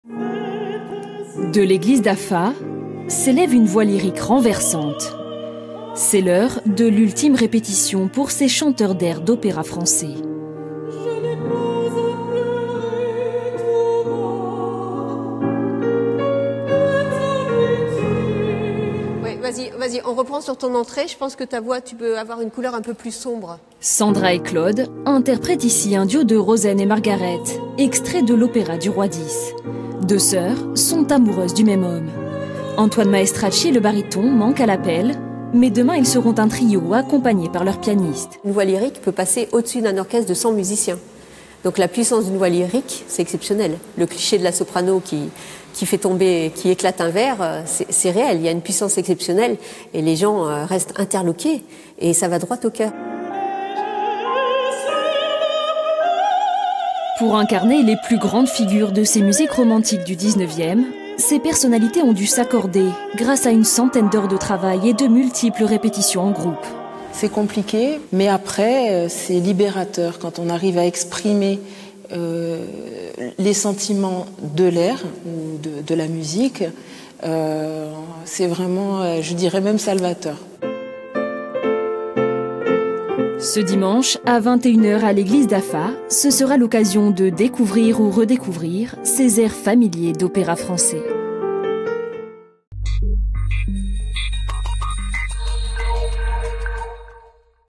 Afa : les plus grands airs de l’opéra français proposés à l’église Saint-Antoine
Dimanche 19 août, à 21 heures, à l'église Saint-Antoine à Afa, est proposé un concert des plus grands airs de l'opéra français. Quatre artistes insulaires y sont réunis.

Intervenants - Martine De Barros, Pianiste ; Claude Gabriel Regis
Soprano.
Equipe - Alexandra Lay ; Jennifer Cappai-Squarcini ; Anastasia Mordin.